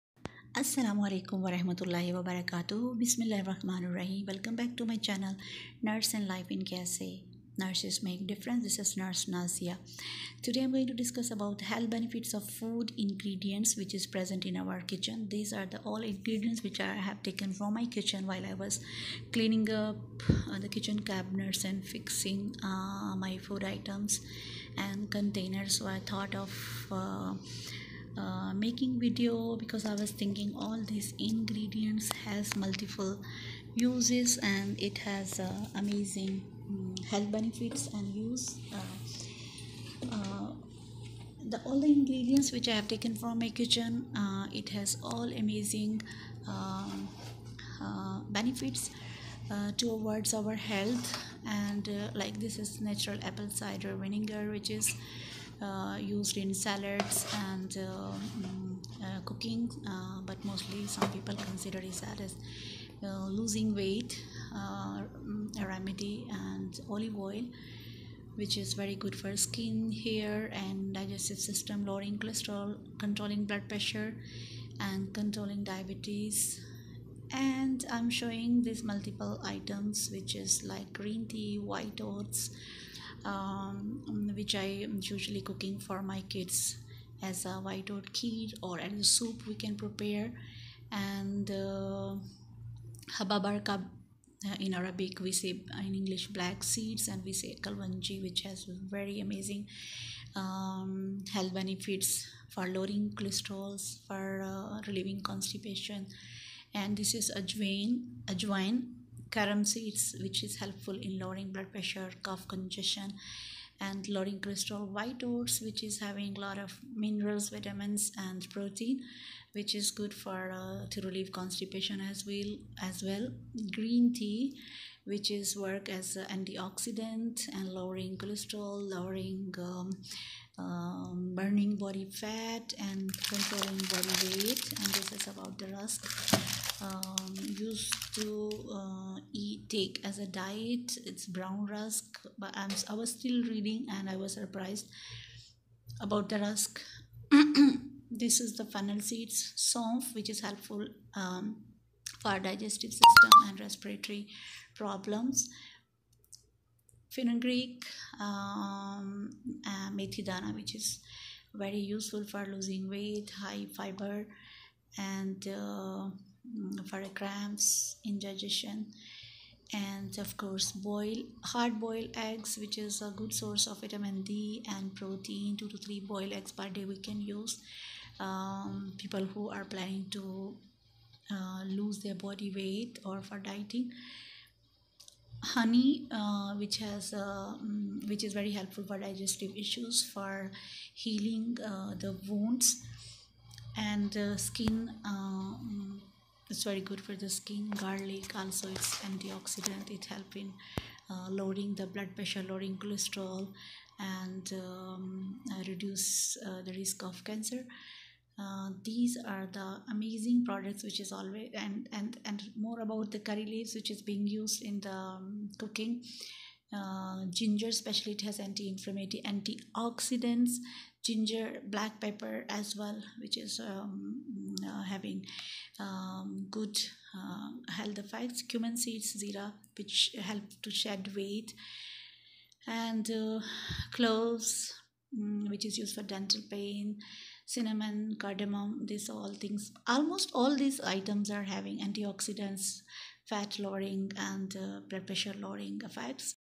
weight loosing remedies| loose weight with ingredients in kitchen| wazan Kam kernay kay formulay.
All ingredients present in our kitchen are following:
1) olive oil. Boost hair, skin health, protect against Suger and insulin resistance and suger. Lower high BP, improves blood cholestroles. Assist I wt lost . Lower risk of osteoporosis.
Black seeds. Antioxidants. Boost sperm count and motility, Increase
 testosterone level in men. Antimicrobial skin, hair problem hair fall. Help In delay gray hair.
Saunf/fennel seeds:used for digestive, resp ailments curing abd cramps, relieve constipation with high dietary fires. Inc intestinal motility, loose weight burn fat.
Cumin seeds /zeera
Noist immunity, anti inflammatory effect, improve digestion, regulate periods, ageless skin, clears up acne, keep hair health. Improve heart health. Shedding body weight.
Fengreek /methi dana
Soluble Fibre, reduce cholesterol, control suger, curbs appitite, Inc metabolism.
Turmeric. Prevent CA, boost immune system. Reduce wt.
Green tea. Reduce bad breath, antioxidant, reduce skin irritation, swelling, fat 🔥.
Ginger.. Soothes Dore muscles, calms nausea, reduce inflammation.
Garlic... Lower BP, cholesterol, Dec bld clot, cure TB, bronchitis, colic, fever, rheumatism.
honey: antioxidant, heal wounds, sooth sore throat, Dec BP, Dec cholestroles, improves sleep quality.